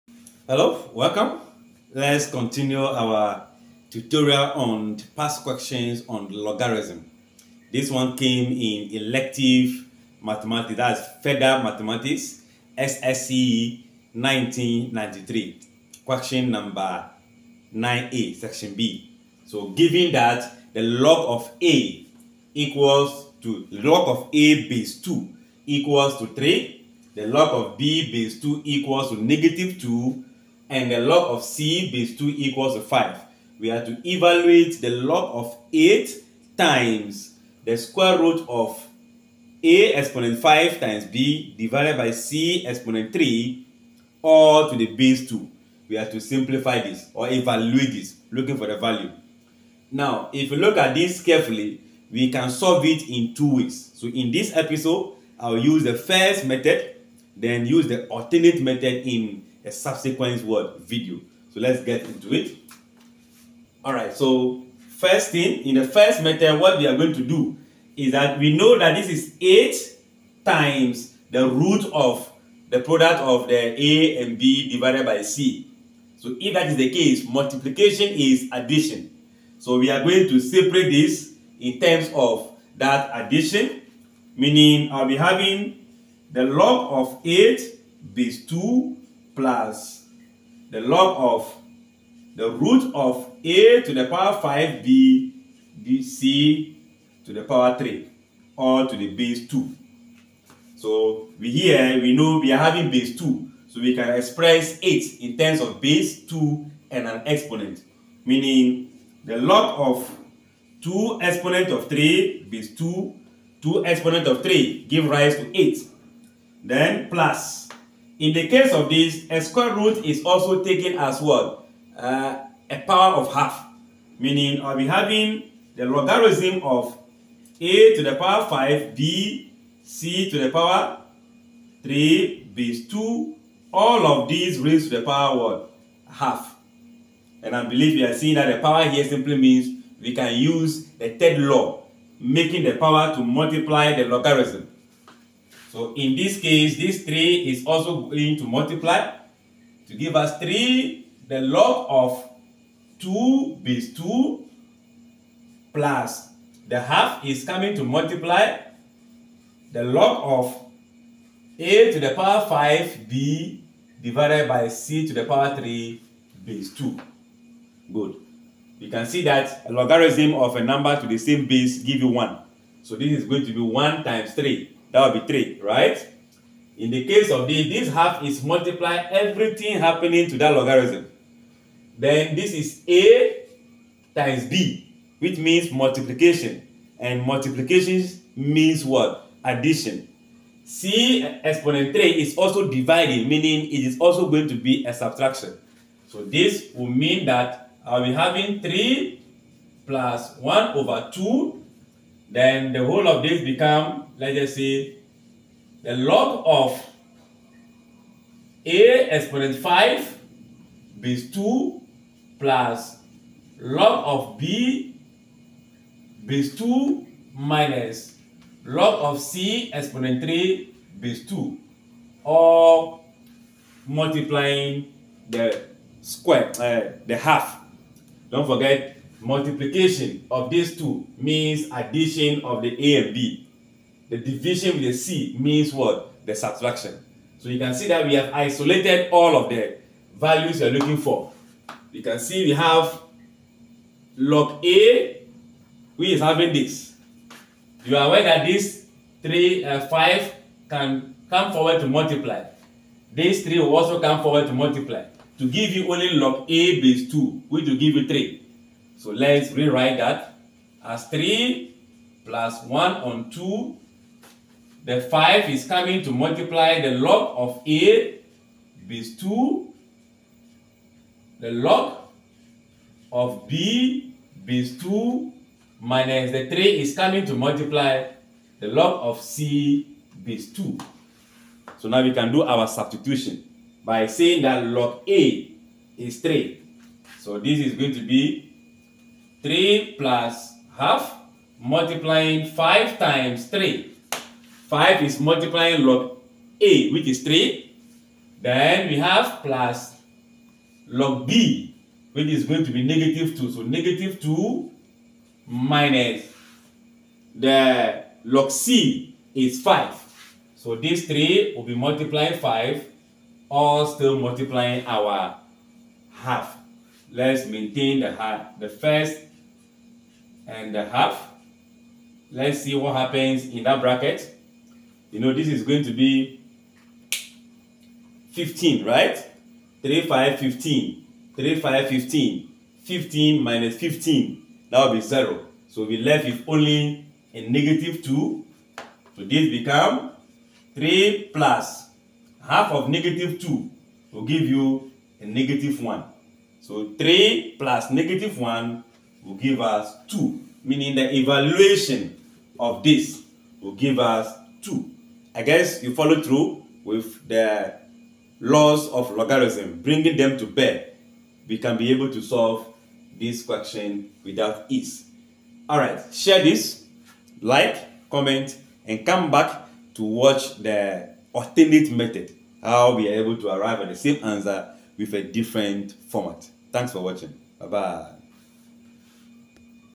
SSSCE 1993 Elective Mathematics: Evaluation of Logarithms
In this video, we will be applying the concept of indices and the laws of logarithms to solve question